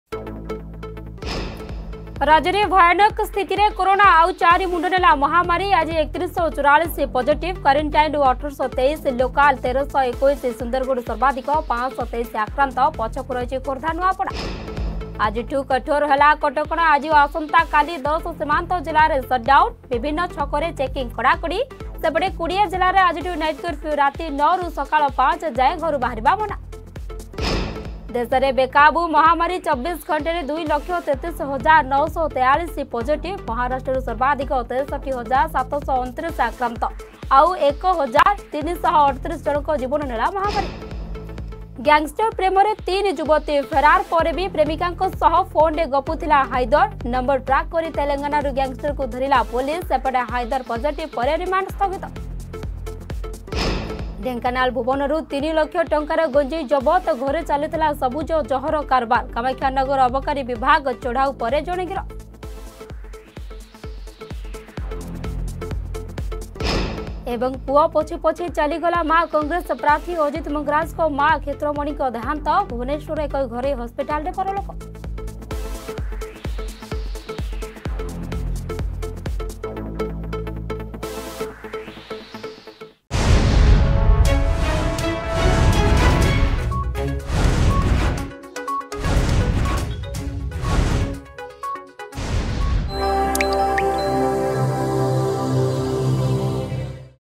HEADLINES 11AM
ରାଜ୍ୟରେ ଭୟଙ୍କର ସ୍ଥିତିରେ କୋରୋନା...
ଆଜିଠୁ କଠୋର ହେଲା କଟକଣା...
ଦେଶରେ ବେକାବୁ ମହାମାରୀ...
ଗ୍ୟାଙ୍ଗଷ୍ଟର୍ ପ୍ରେମରେ ତିନି ଯୁବତୀ
ଢେଙ୍କାନାଳ ଭୂବନରୁ 3 ଲକ୍ଷ ଟଙ୍କାର ଗଞ୍ଜେଇ ଜବତ...
ପୁଅ ପଛେ ପଛେ ଚାଲିଗଲା ମା...